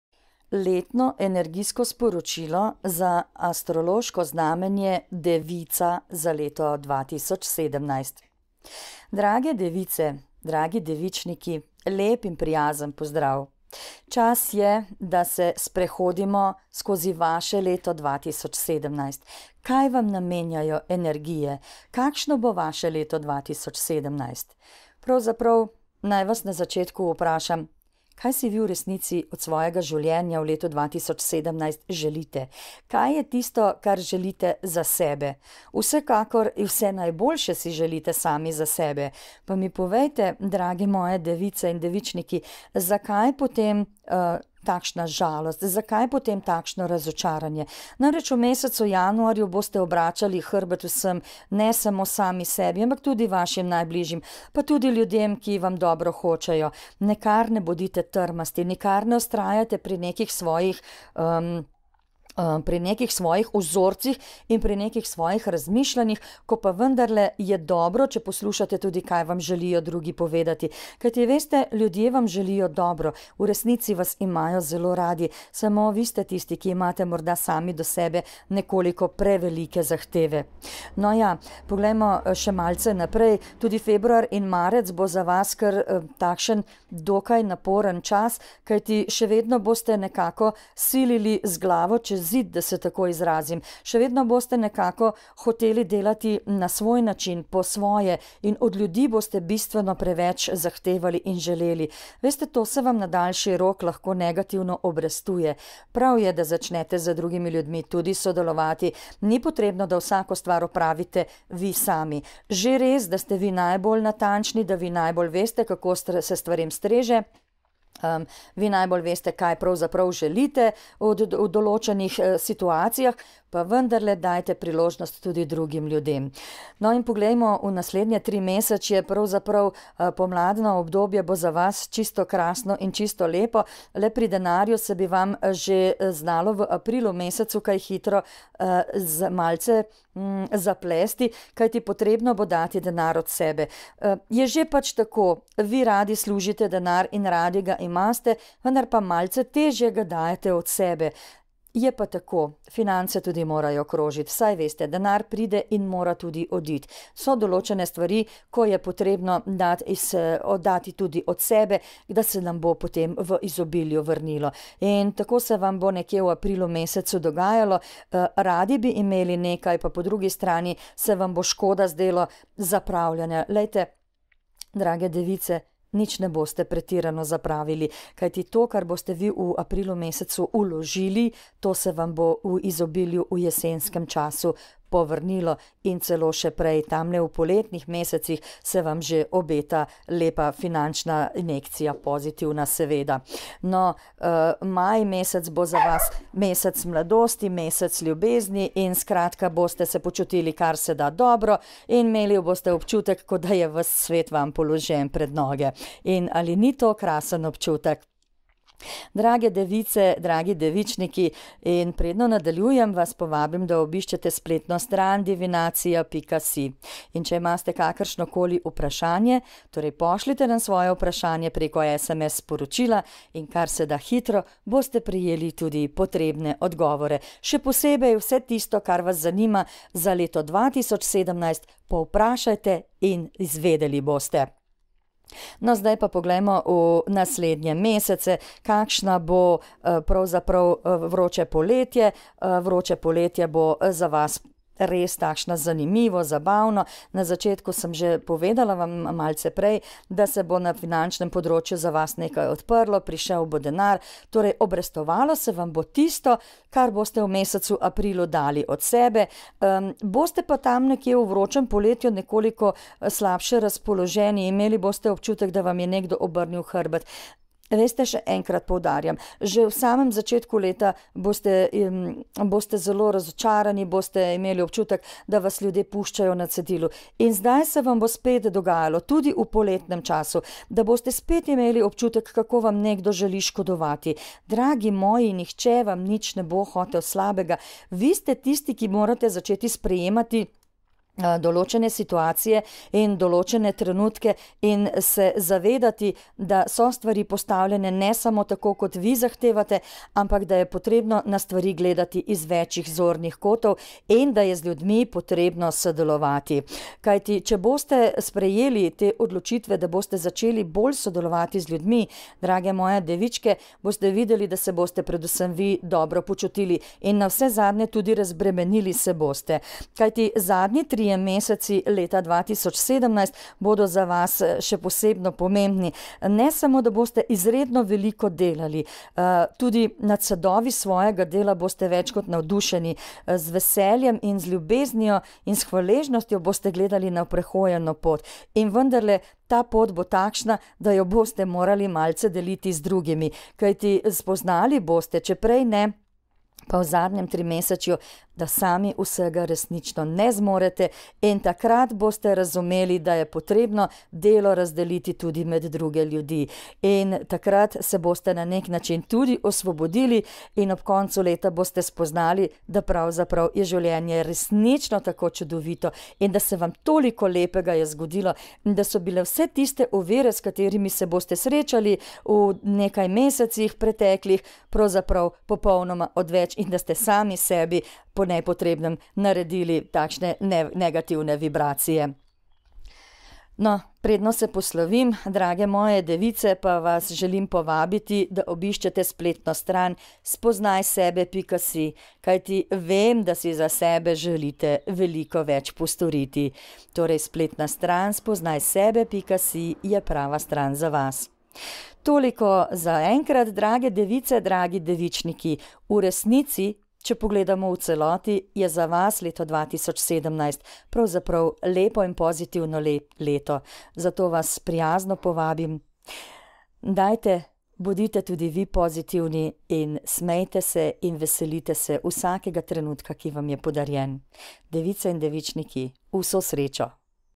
Letni horoskop Devica 2017
Poslušajte vaš letni horoskop 2017 za astrološko znamenje Devica.